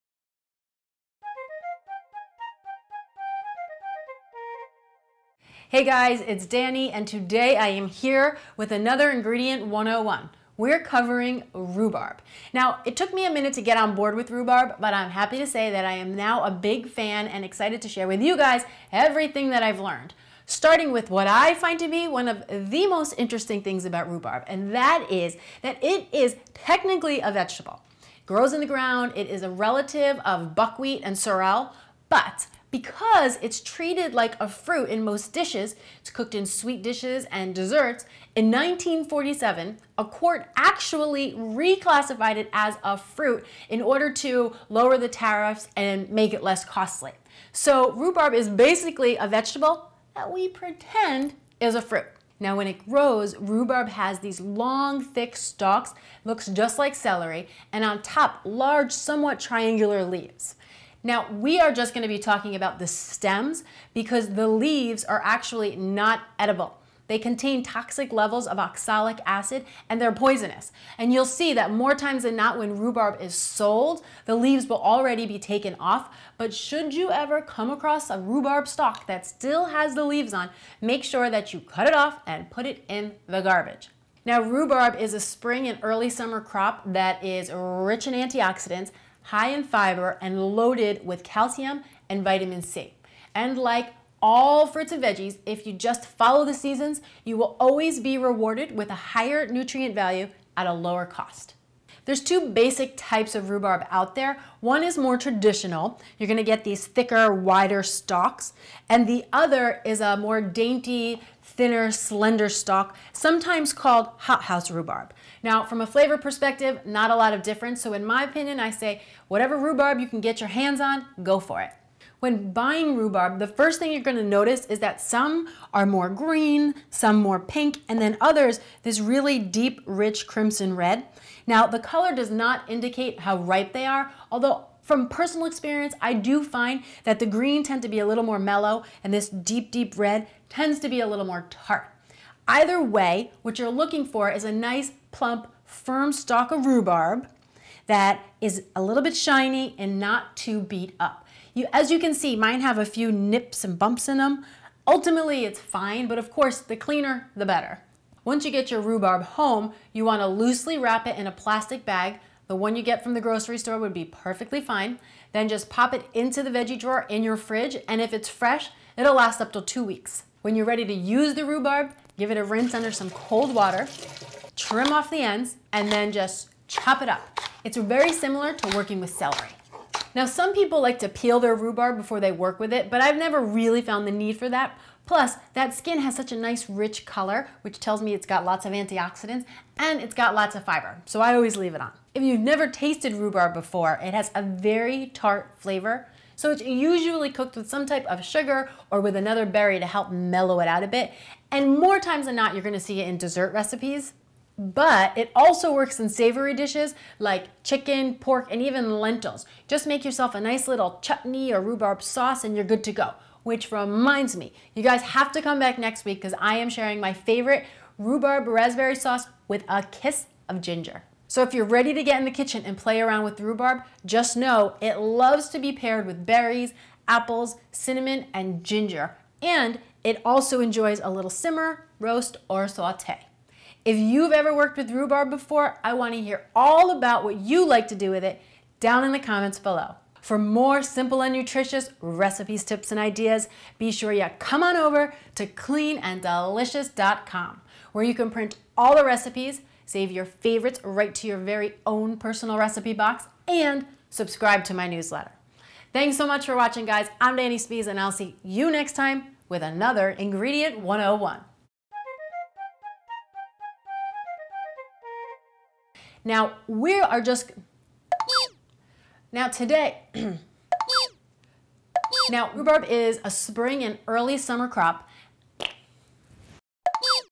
Rhubarb 101 - Everything You Need To Know About Rhubarb
Learn everything you need to know about buying, storing, and preparing rhubarb in this short, instructional video.

And be sure to check back next week for a recipe for Strawberry Rhubarb Sauce (which is great on top of pancakes!).